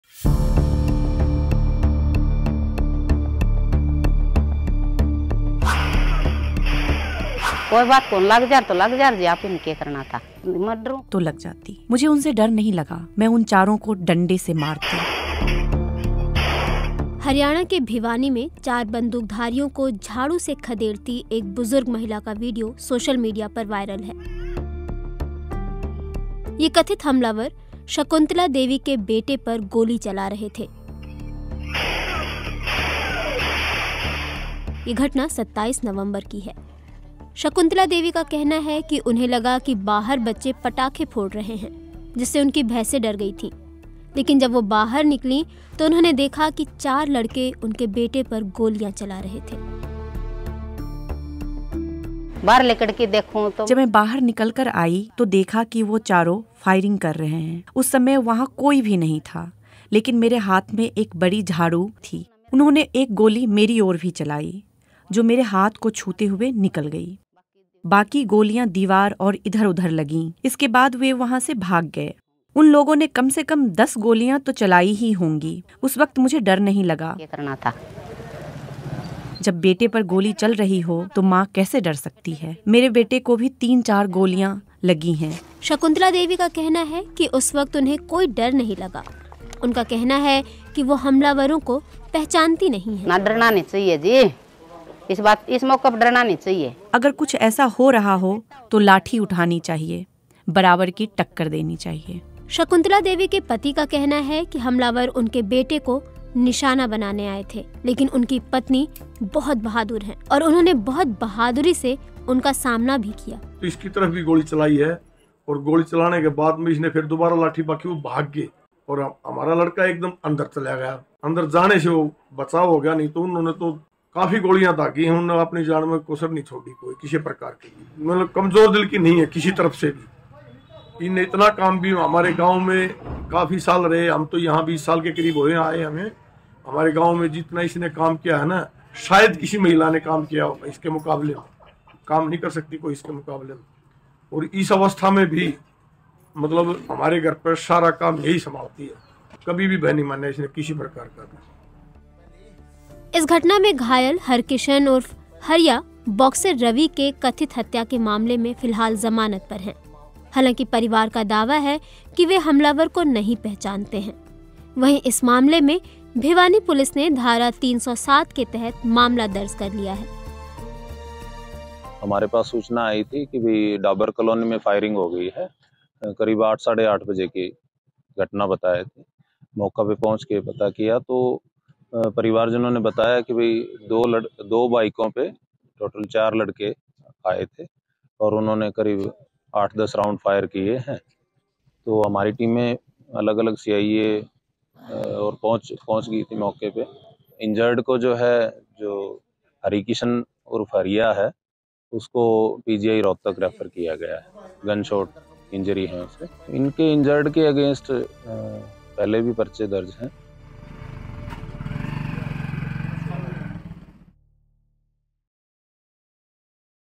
Haryana brave mother: बेटे पर हो रही थी फ़ायरिंग, तभी मां झाड़ू लेकर आ पहुंची (BBC Hindi)
हरियाणा के भिवानी में चार बंदूकधारियों को झाड़ू से खदेड़ती एक बुज़ुर्ग महिला का वीडियो सोशल मीडिया पर वायरल हो रहा है. ये कथित हमलावर शंकुतला देवी के बेटे पर गोली चला रहे थे. ये घटना 27 नवंबर की है. शकुंतला देवी का कहना है कि उन्हें लगा कि बाहर बच्चे पटाखे फोड़ रहे हैं. जिससे उनकी भैंसें डर गईं. लेकिन जब वो बाहर निकलीं तो उन्होंने देखा कि चार लड़के उनके बेटे पर गोलियां चला रहे थे. देखिए, उसके बाद क्या हुआ.

रिपोर्ट : सत सिंह
एडिट : राजन पपनेजा